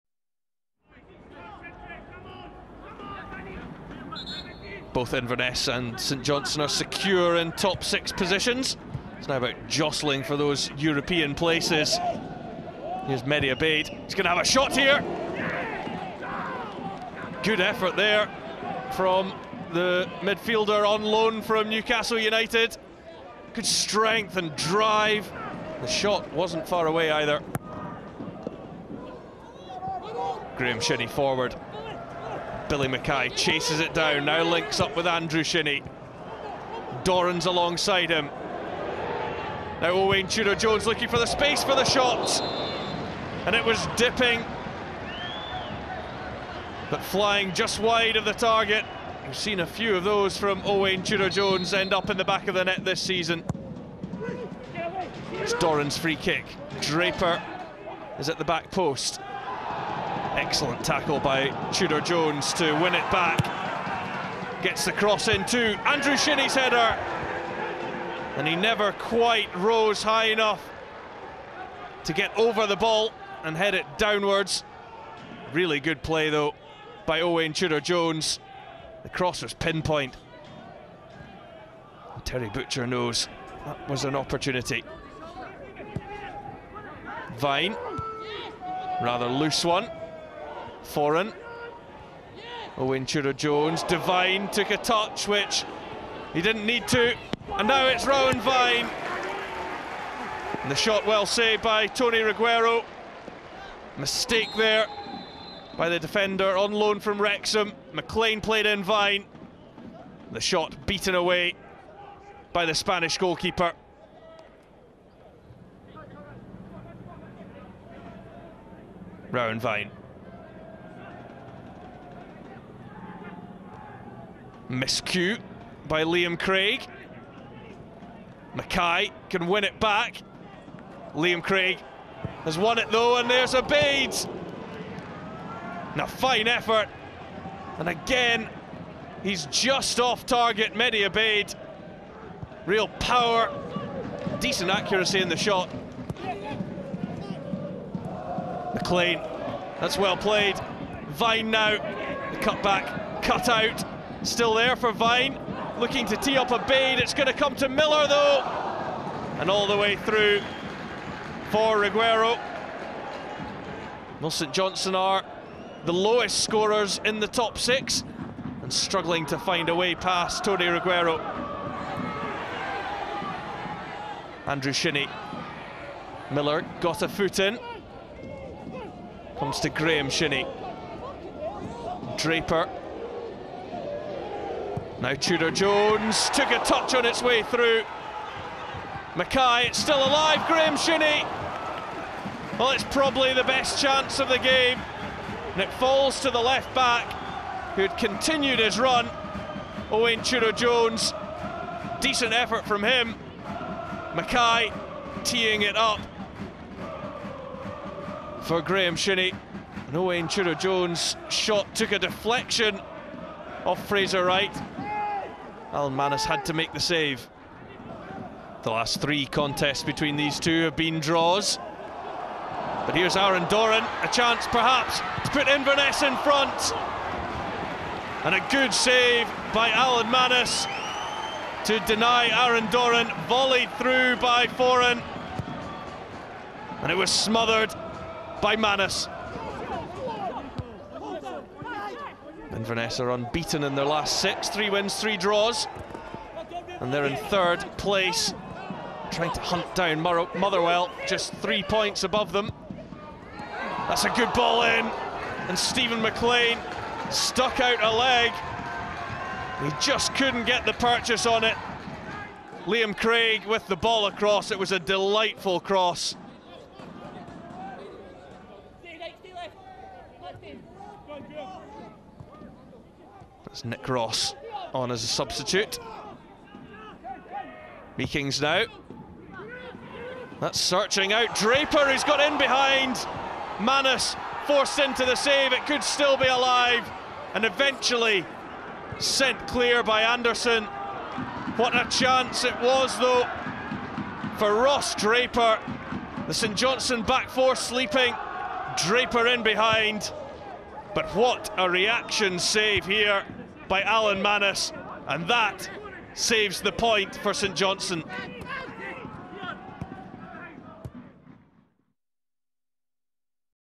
Inverness CT 0-0 St Johnstone, 05/04/2013
Inverness CT consolidated third place in the SPL table after holding challengers St Johnstone to a 0-0 draw at the Caledonian Stadium.  Most of the best chances fell to the home side but Saints keeper Alan Mannus was in great form, producing a series of fine saves to frustrate Terry Butcher's men.